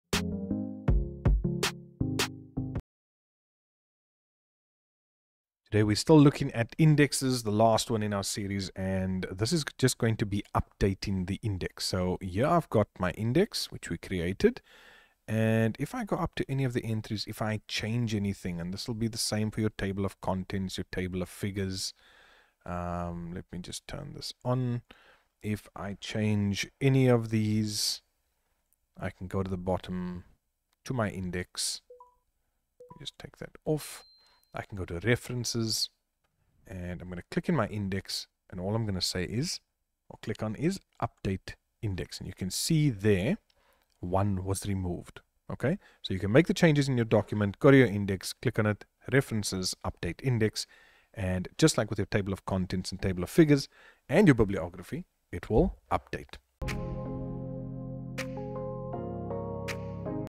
How to UPDATE your INDEX in MS WORD
In a previous we looked at HOW to insert an Index. Today we are going to look at how to UPDATE the INDEX in MS WORD.